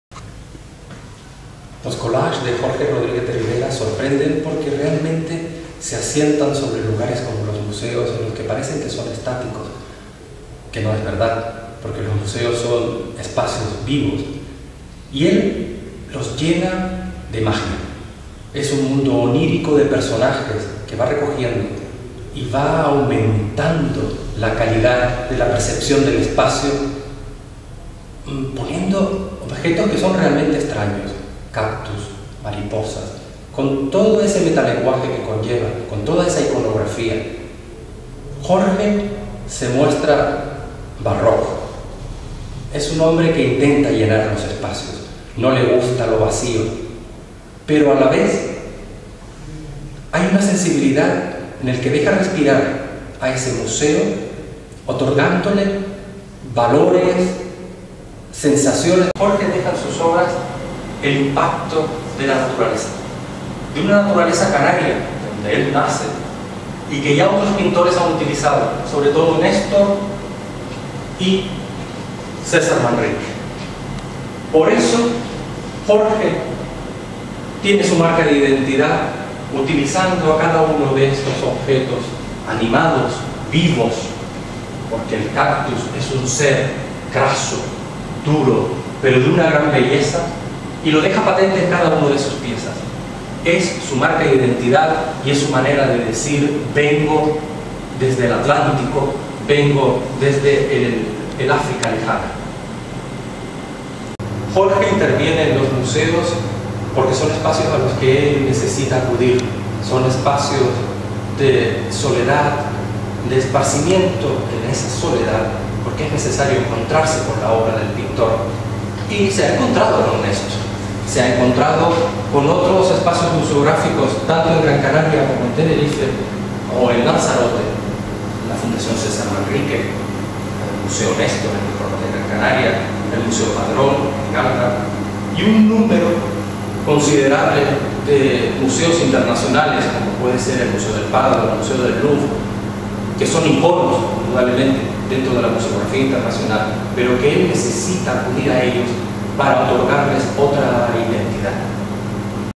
Los collages de Jorge Rodríguez de Rivera comentados por Daniel Montesdeoca
Daniel Montesdeoca(Director del Museo Nestor de Las Palmas de Gran Canaria) comenta los collages de Jorge Rodríguez de Rivera con motivo de su exposición en TEA (Tenerife) del 9 de julio al 6 de septiembre del 2015 : A propósito de Oscar